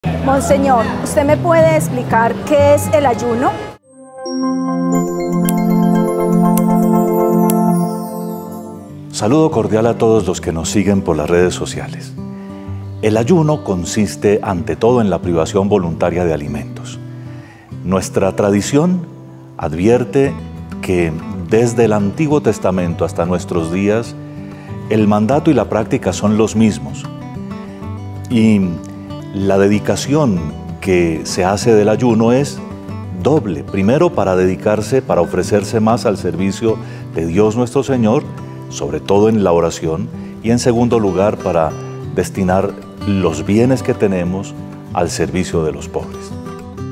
Qué es el Ayuno - Monseñor José Miguel Gómez, Arzobispo de Manizales
¿Qué es el ayuno? ¿Cómo se hace el ayuno? El Obispo responde. Monseñor José Miguel Gómez Rodríguez, Arzobispo de Manizales, nos explica todo sobre el ayuno.

En qué consiste el ayuno. El ayuno en la Biblia. Qué dice la tradición sobre el ayuno. Para qué se hace el ayuno. ¿Por qué privarnos de los alimentos? ¿Quienes deben ayunar? ¿Cómo ayunar?